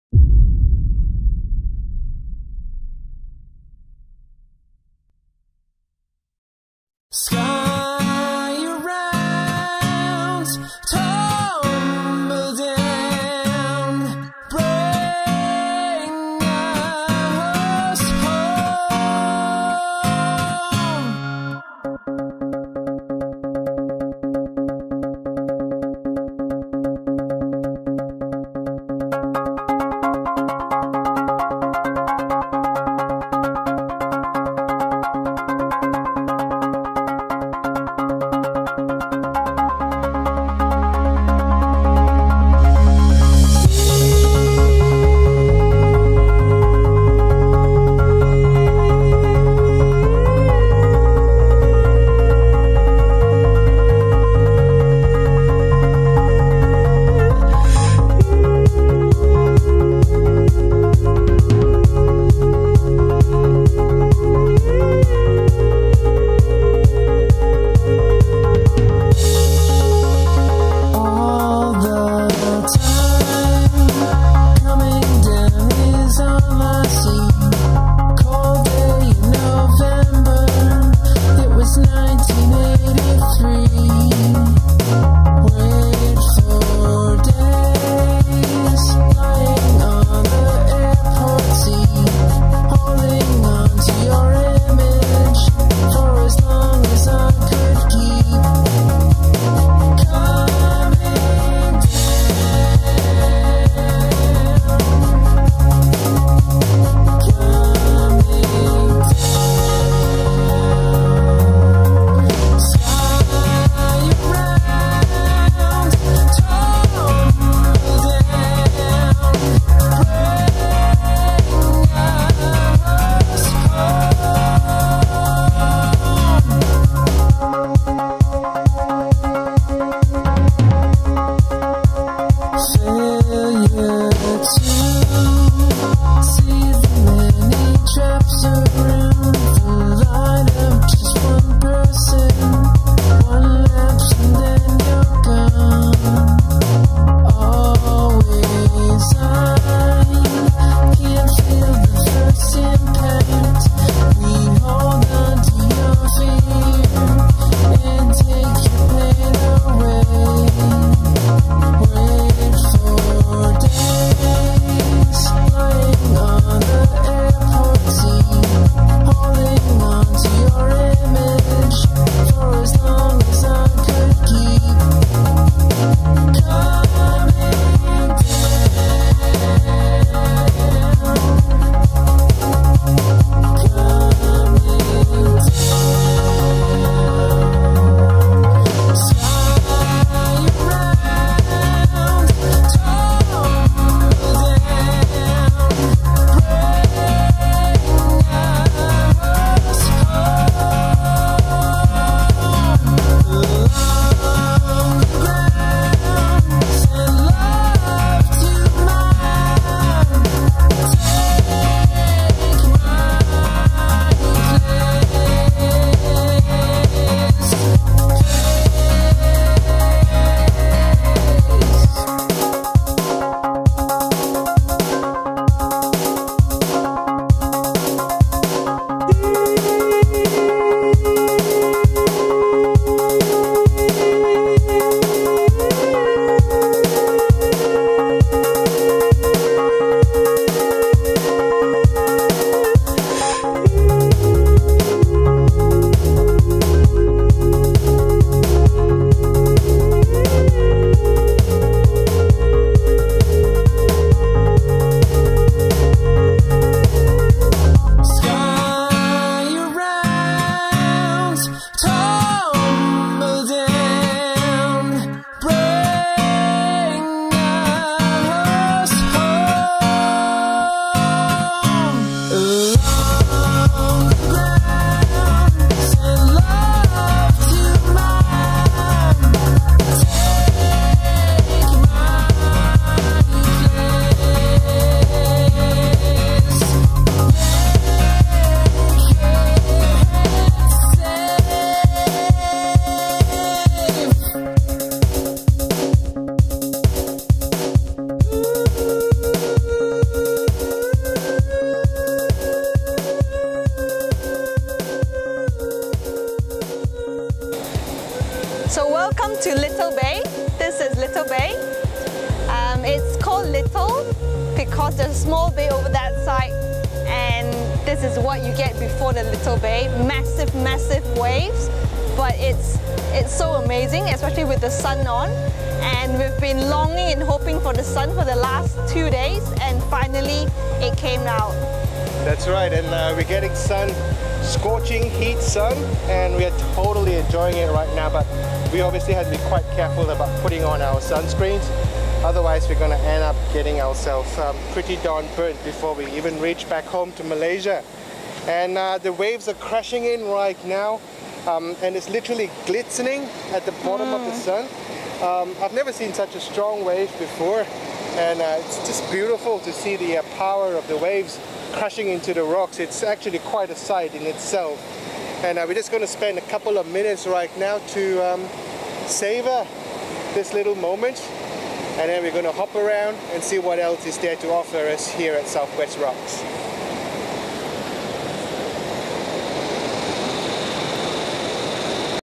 Caravan trip from Sydney to Brisbane
My last blog entry for Australia. This time a caravan trip from Sydney to Brisbane. The perfect wrap-up for such an amazing and epic time in the land down under.